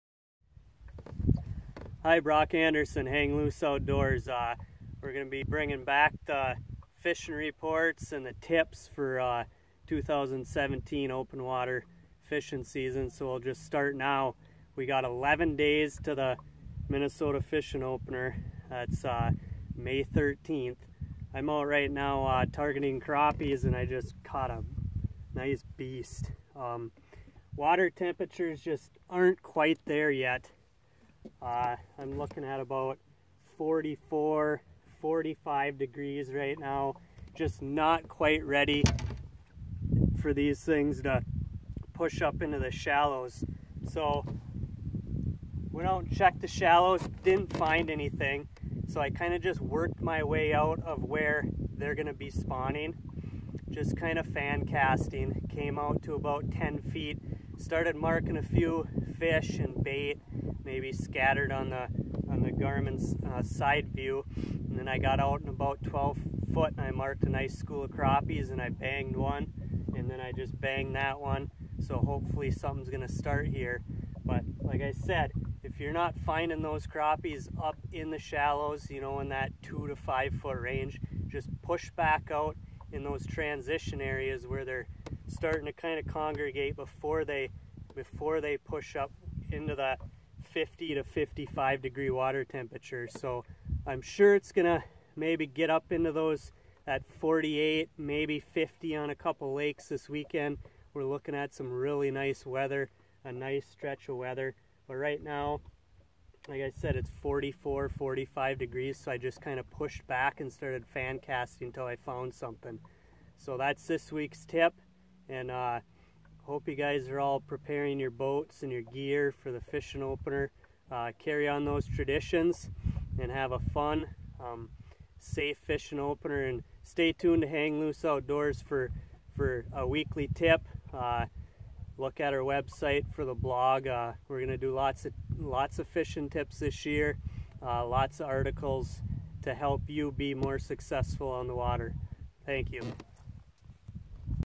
Grand Rapids, MN Fishing Report May 2, 2017
HLO crappie fishing tip. Brock explains how to target crappies when they are not quite ready to be in their shallow spawning areas.